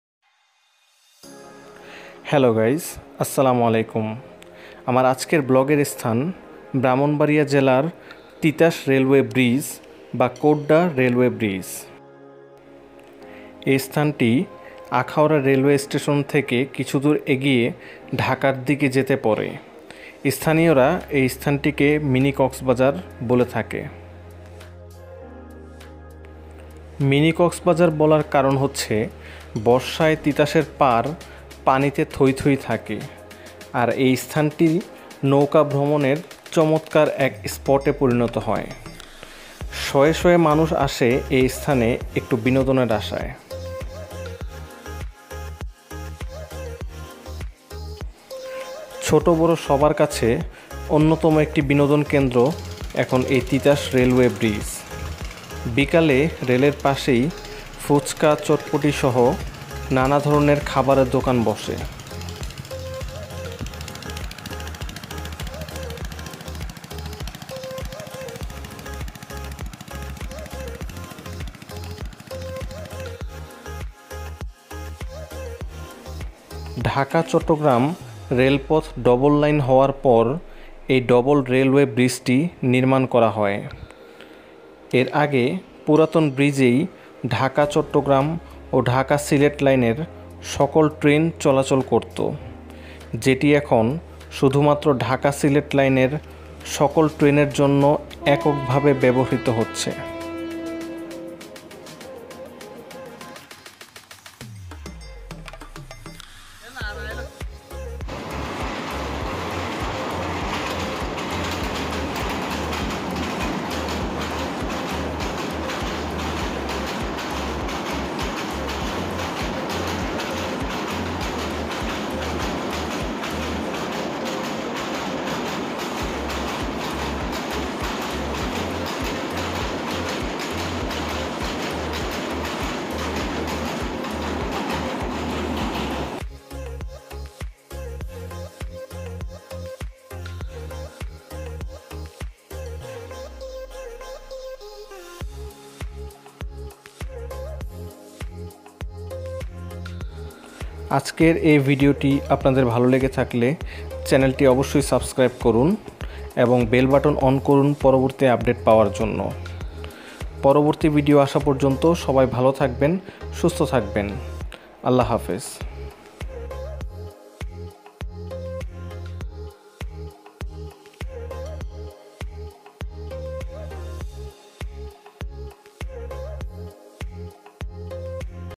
Akhaura Mini Cox's Bazar | তিতাস নদীর রেলওয়ে ব্রীজ | Akhaura Brahmanbaria | Vlog-46
এই রেলওয়ে ব্রিজ দুটি আখাউড়া উপজেলার কোড্ডায় অবস্থিত। এটি একমাত্র রেলওয়ে ব্রিজ যা ঢাকা-সিলেট ও ঢাকা-চট্রগ্রামের রেলপথ বিভক্ত হয়েছে। আখাউড়া উপজেলা ও আশপাশের মানুষের অন্যতম বিনোদনের স্থান এখন এই কোড্ডা রেলওয়ে ব্রিজ এলাকা।